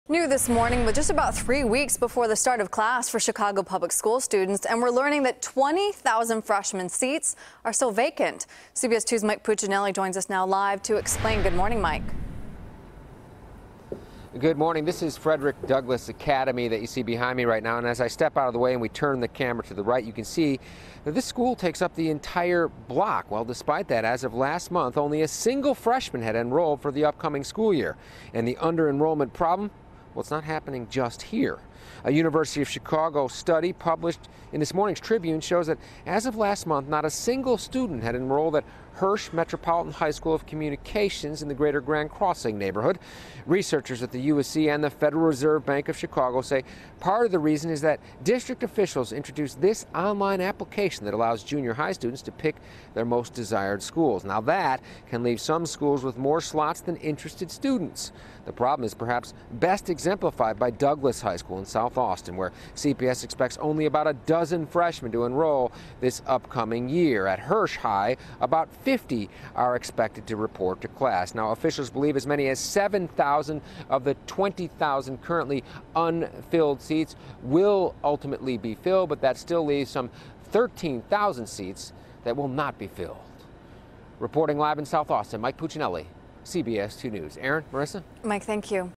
Report: 20,000 Freshman Seats Still Vacant At Chicago Public Schools
With less than three weeks before the start of class for Chicago Public Schools students, approximately 20,000 freshman seats are still vacant at district high schools. CBS 2's Mike Puccinelli reports.